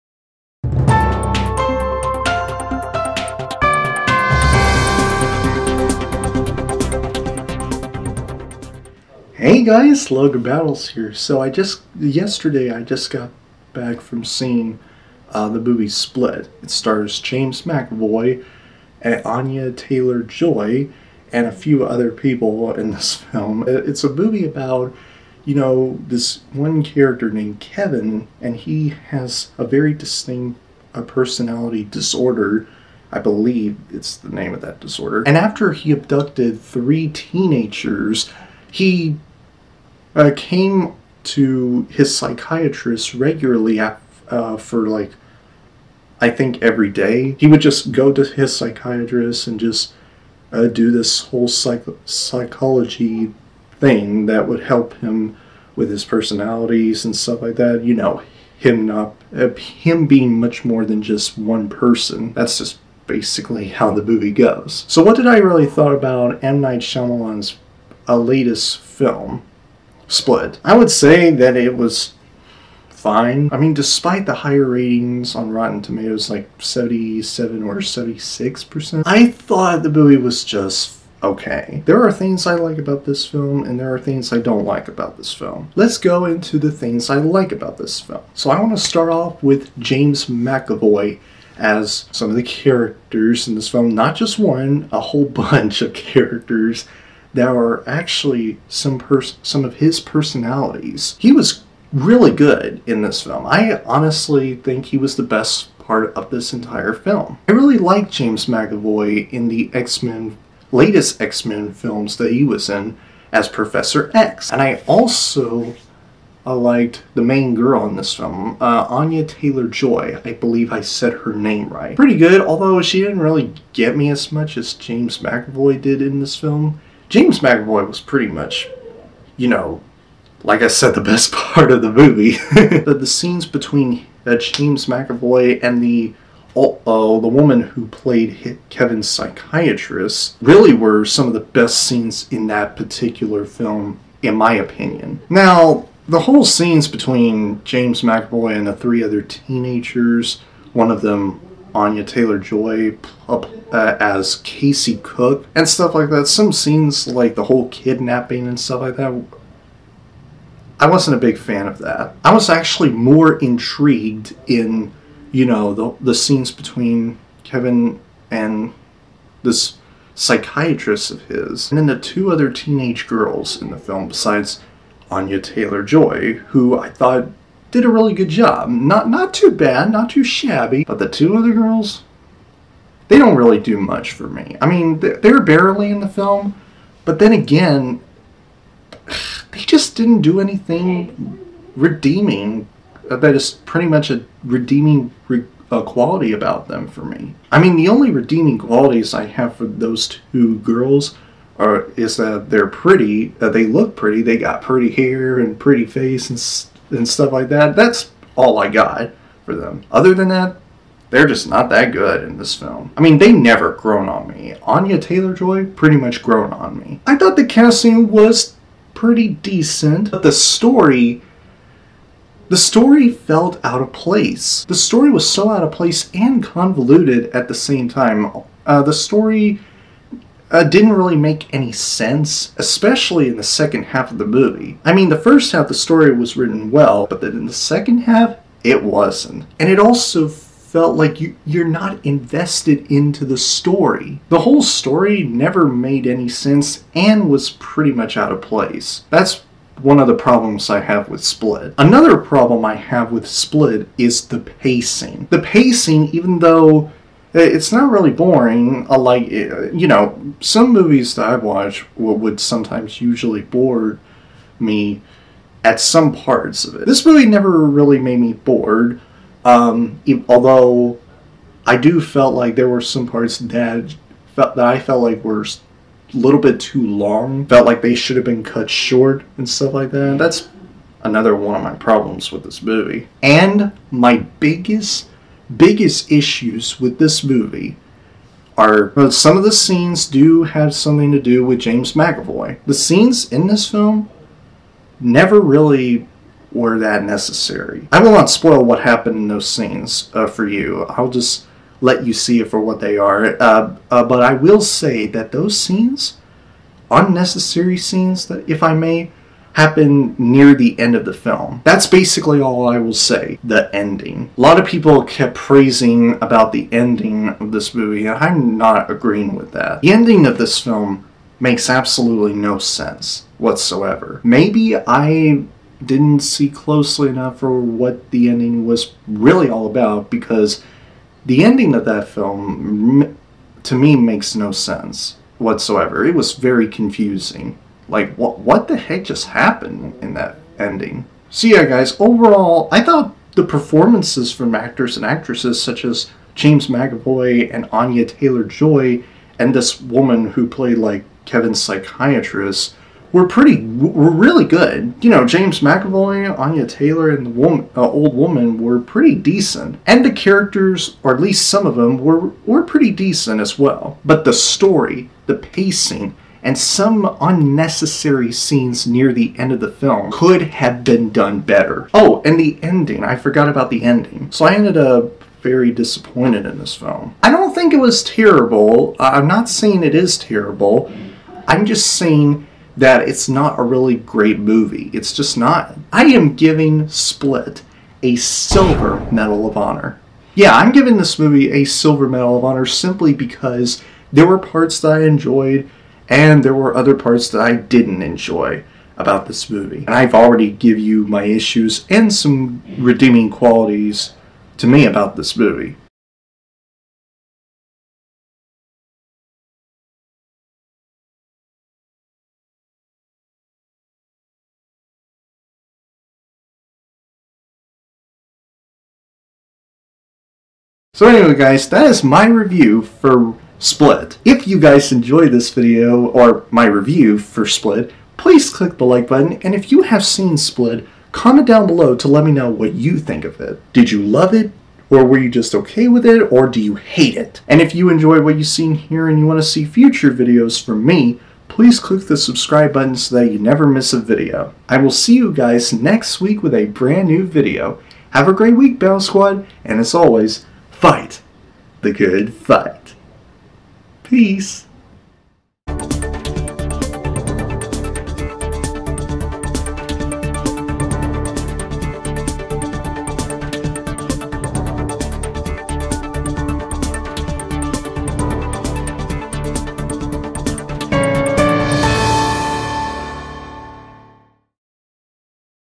Split - Movie Review
For my review for one of my most anticipated movies of the year and the first 2017 movie I have reviewed this year, I get into what are the things I liked and disliked about the latest M. Night Shyamalan film, Split.

Fight The Good Fight, Battle Squad!